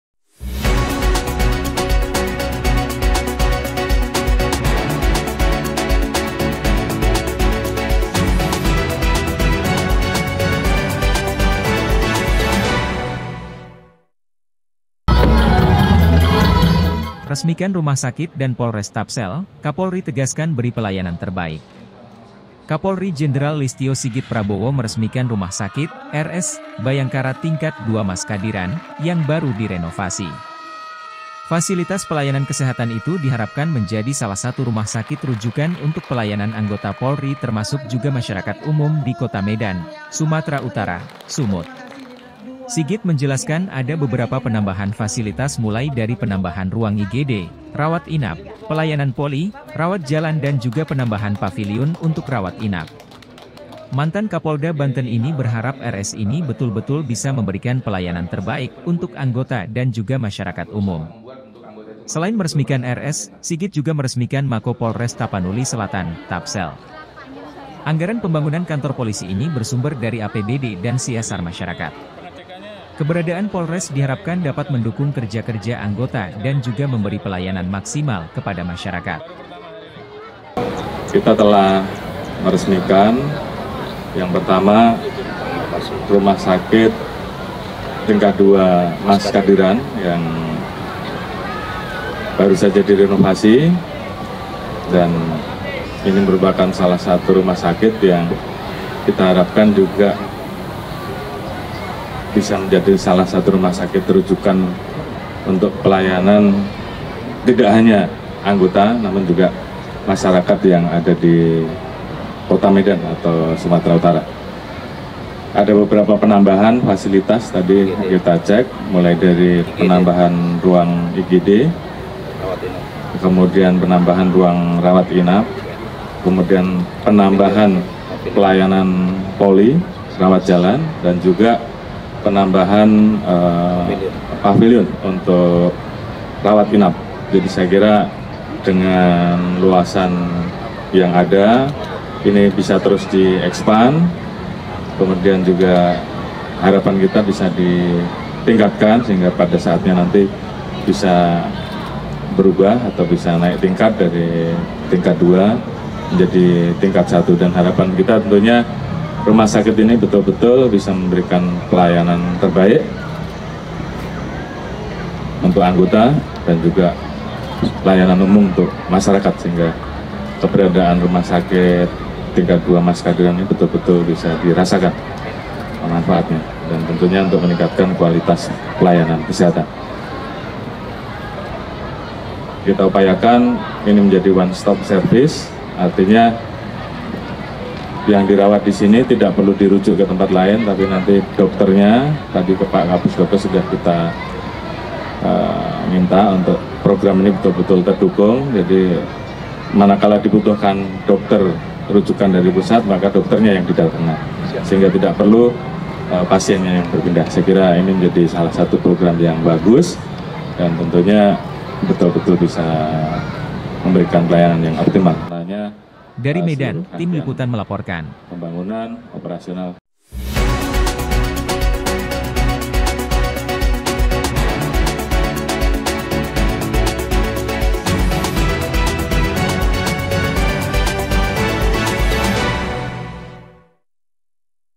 RESMIKAN RUMAH SAKIT DAN POLRES TAPSEL, KAPOLRI TEGASKAN BERI PELAYANAN TERBAIK
Kapolri Jenderal Listyo Sigit Prabowo meresmikan Rumah Sakit (RS) Bhayangkara tingkat II Mas Kadiran, yang baru di renovasi. Fasilitas pelayanan kesehatan itu diharapkan menjadi salah satu rumah sakit rujukan untuk pelayanan anggota Polri termasuk juga masyarakat umum di Kota Medan, Sumatera Utara (Sumut).

Sigit menjelaskan ada beberapa penambahan fasilitas mulai dari penambahan ruang IGD, rawat inap, pelayanan poli, rawat jalan dan juga penambahan paviliun untuk rawat inap.

Mantan Kapolda Banten ini berharap RS ini betul-betul bisa memberikan pelayanan terbaik untuk anggota dan juga masyarakat umum.

Selain meresmikan RS, Sigit juga meresmikan Mako Polres Tapanuli Selatan (Tapsel). Anggaran pembangunan kantor polisi ini bersumber dari APBD dan CSR masyarakat. Keberadaan polres diharapkan dapat mendukung kerja-kerja anggota dan juga memberi pelayanan maksimal kepada masyarakat.

JOURNALIST : SUPRI YADI
EDITOR : IQBAL